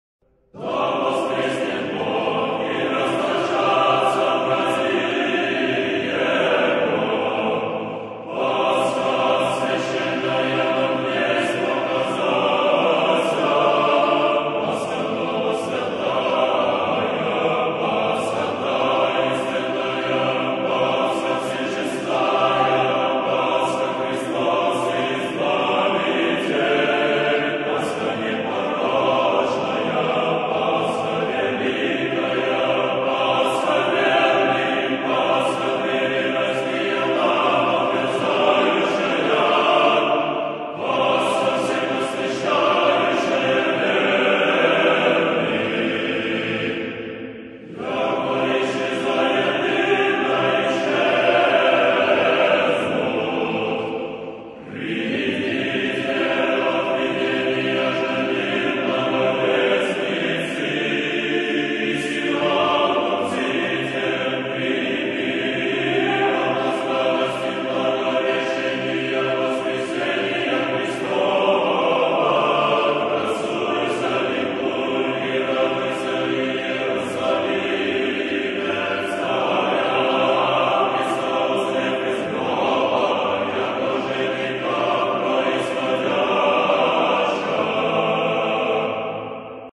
Orthodox Metropolitan of Kiev and all Ukraine Onufry - on the Crete's Council
HIs High Eminance, Metropolitan of Kiev and all Ukraine Onufry about the Pan-Orthodox Council.